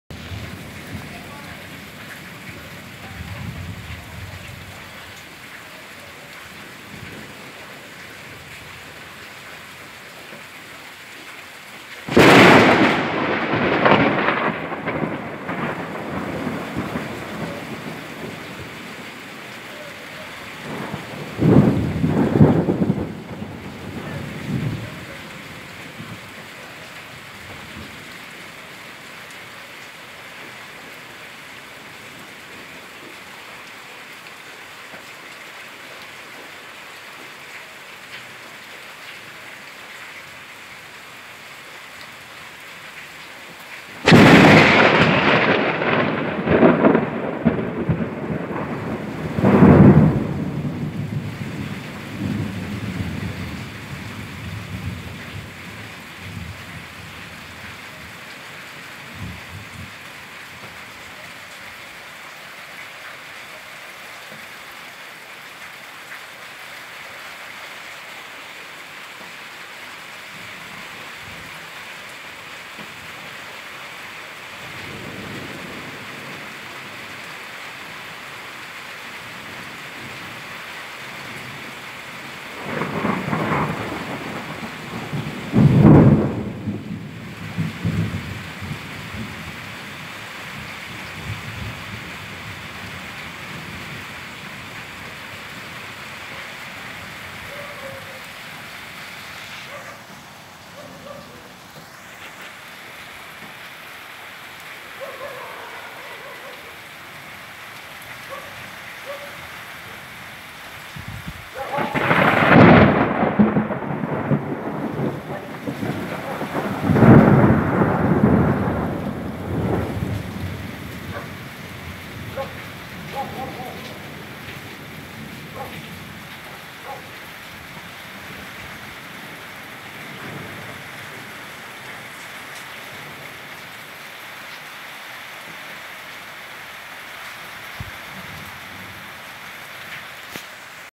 Tormenta electrica
Una noche de lluvia en la sierra mixe 2018